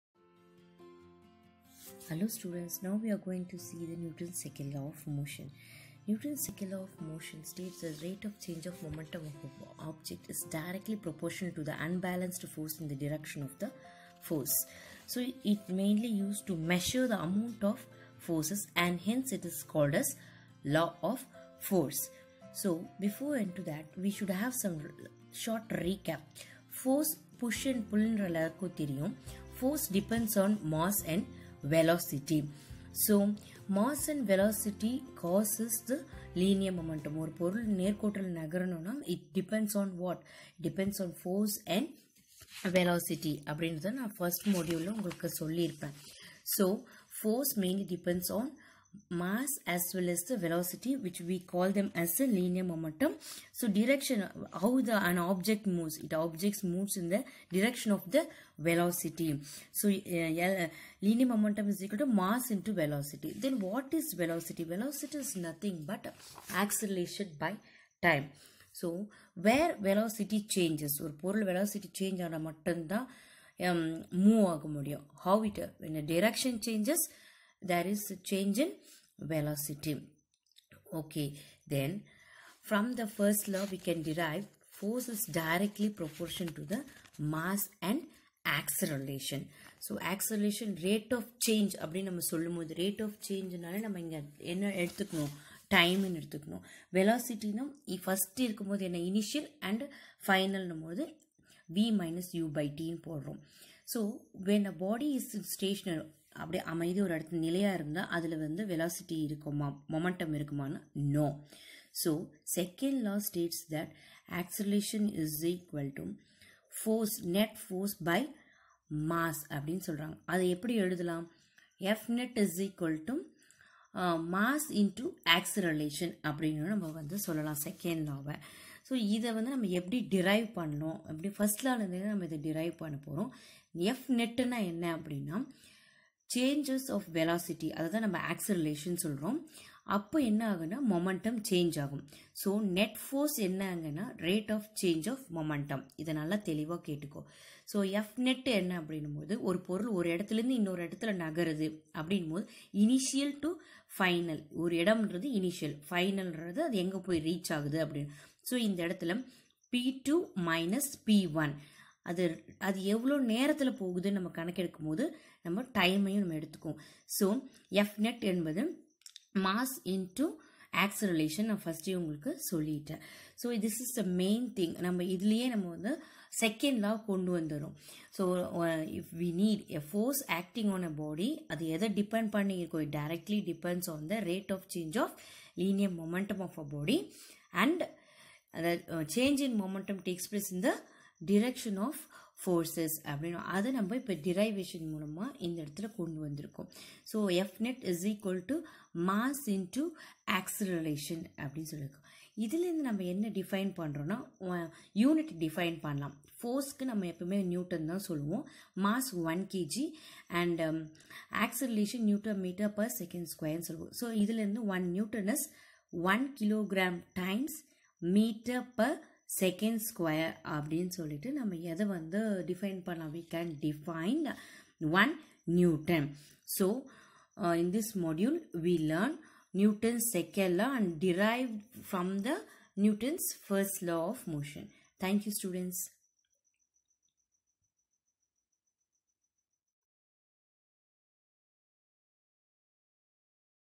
10th Science /Newton's Second Law of Motion
This video about 10th Science Newton's Second Law of Motion